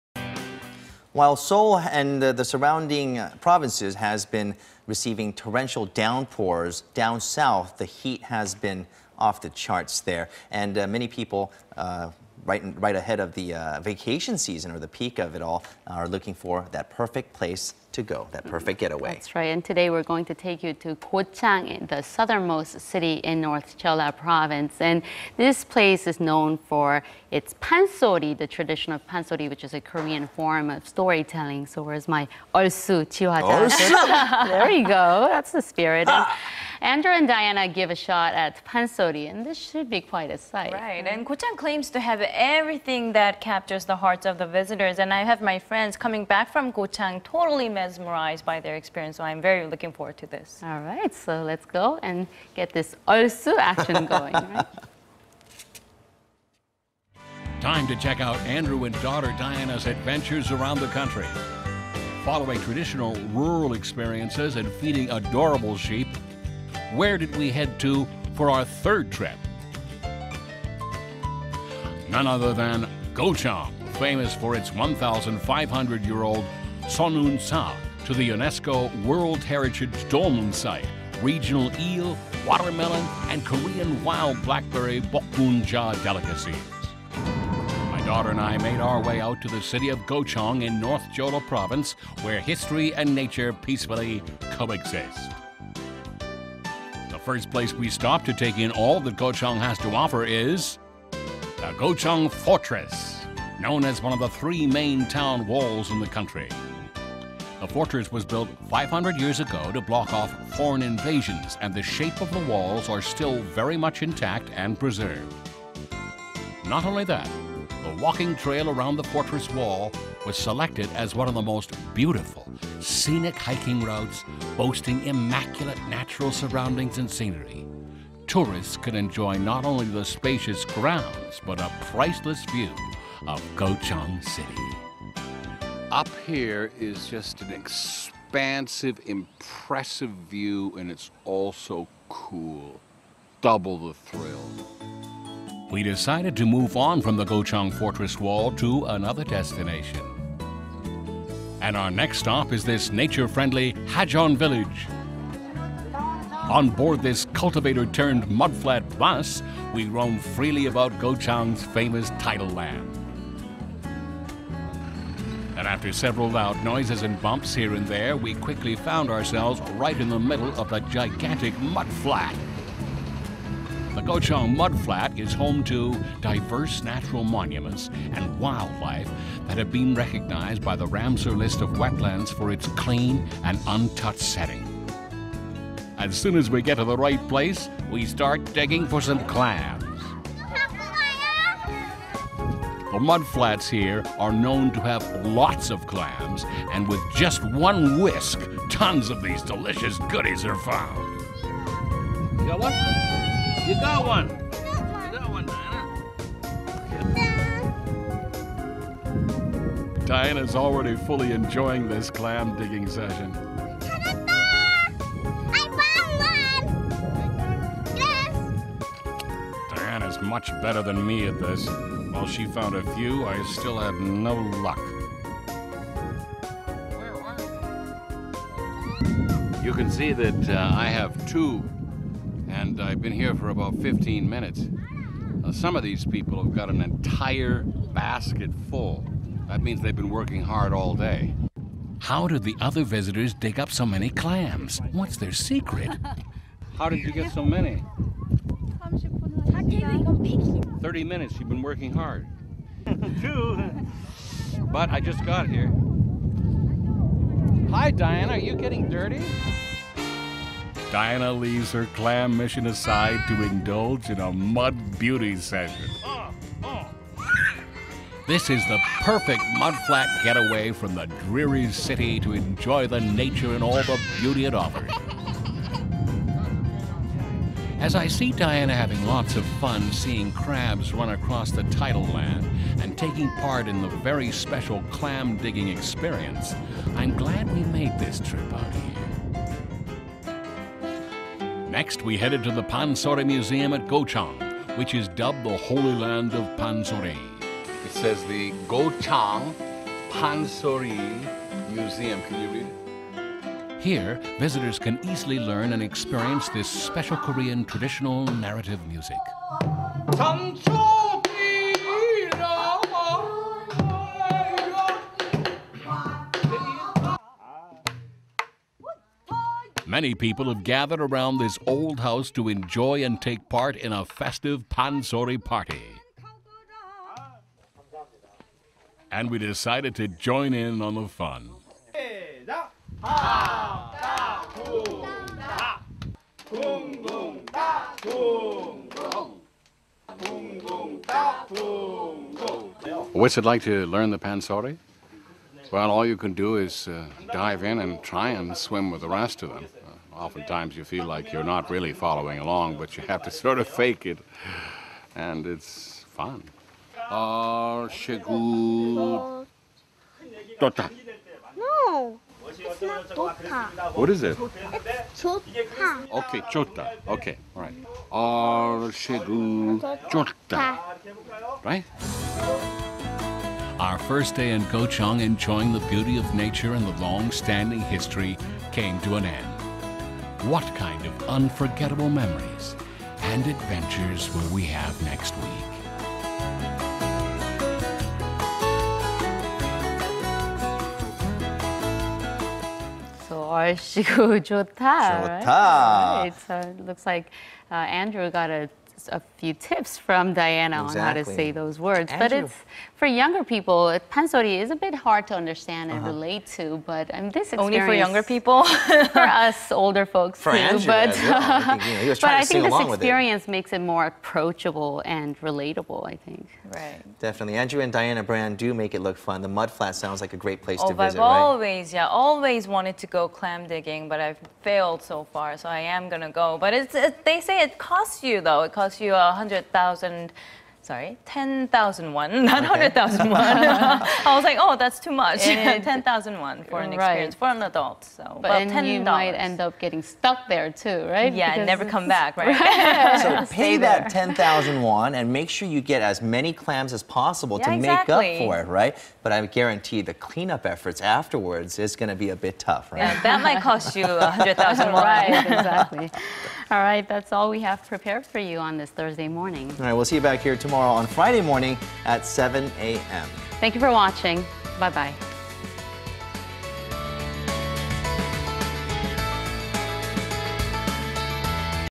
DIANA ELIZABETH BRAND AND ANDREW BRAND IN GOCHANG
Diana Elizabeth Brand and her daddy Andrew William Brand visit the Pansori capital of Korea. (p'ansori, sometimes called Korean folk opera, puk [Credit: Wesleyan University Virtual Instrument Museum a genre of narrative song of Korea, typically performed dramatically by a vocalist, accompanied by a puk (double-headed barrel drum). Built from the word p'an, meaning "open space," and sori, meaning "singing" or "sound," the term p'ansori itself is a reference to the markets, public squares, and other such open venues where performances originally took place.)